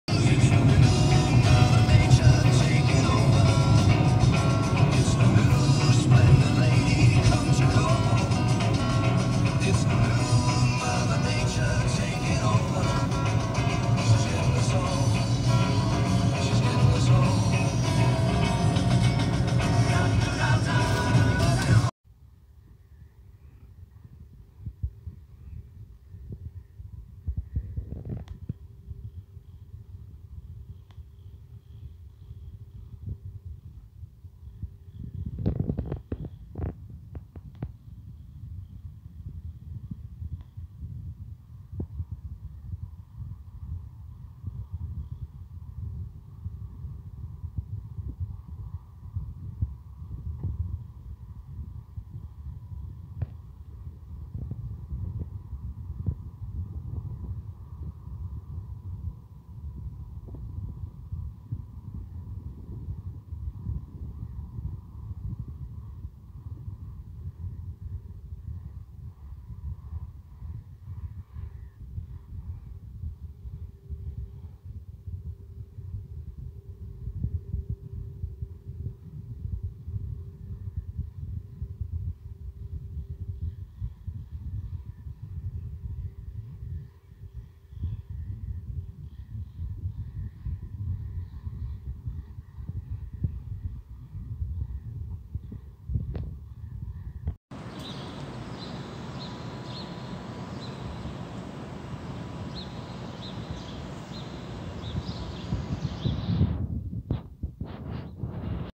Winnipeg Stadium, IG Field, short view of outside. Winnipeg Manitoba, Canada
View driving to IG Field in Winnipeg, Manitoba. Short walk around Stadium near team store. Good views through fence into Stadium.